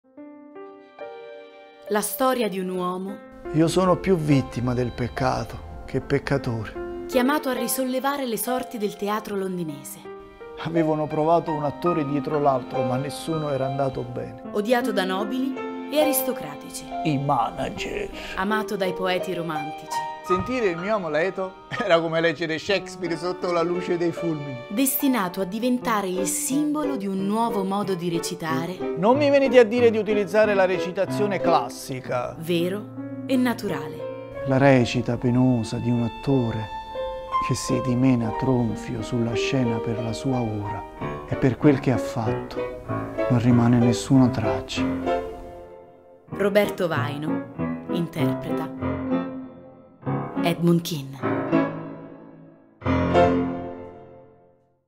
EDMUND KEAN con Roberto Vaino - Trailer
“E’ un Edmund Kean napoletano che racconta la sua storia utilizzando anche citazioni di Shakespeare”.

Farti sorridere, riflettere e sognare. Questa è la sfida di Roberto Vaino.

La scelta del napoletano vuole essere un mezzo per raccontare in modo nuovo la vita del più grande attore shakespeariano di tutti i tempi, simbolo del Teatro Reale Drury Lane… chiedendo al pubblico, quindi, uno sforzo di immaginazione perché non ambientato a Napoli ma a Londra, nell’Inghilterra del 1800. La vita di quest’uomo è una mescolanza di dramma e ironia/leggerezza. Come quando non potendo pagare la carrozza da Birmingham a Swansea, percorse a piedi 12 km al giorno insieme alla sua famiglia, dormendo all’aperto nei campi per poi raccontare, qualche anno dopo, che “in realtà in estate si dorme meglio all’aperto nei campi”.

Le citazioni di Shakespeare sono oggi più che mai vive e palpabili… Roberto racconta spesso che quando ripete il suo monologo ad alta voce per strada… le persone lo fermano e gli dicono “che belle parole, dovresti scriverle…” e lui risponde “magari.. sono state già scritte 400 anni fa.

Liberamente tratto dal libro “Edmund Kean, Fire From Heaven” di Raymund FitzSimons.
Con Roberto Vaino.
Regia di Roberto Vaino e con la collaborazione di Chiara D’Anna.
Luci di Giovanni Samaritani.
Musiche di Michele Biki Panitti.
Costumi di sorelle Garofalo.
Fotografia di Laura De Meo.
Voce Trailer di Viola Iacobelli.
Acting Coach: Cinzia Alitto, Chiara D’Anna ed Enzo Samaritani.